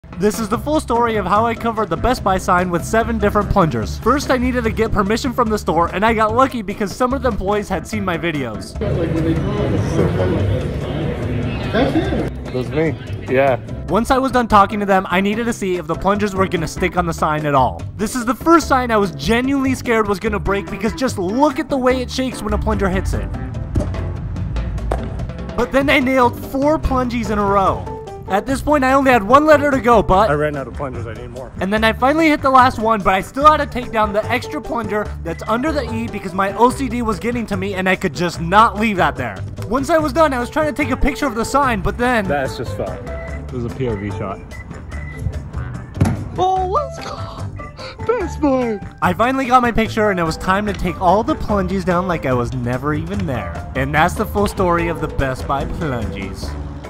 How Best Buy Let Me Put 7 Plungers On Their Sign 🤽🏼‍♂️🪠
This is the story of how Best Buy let me put 7 plungers on their sign.

Follow these if you are cool (I know you are)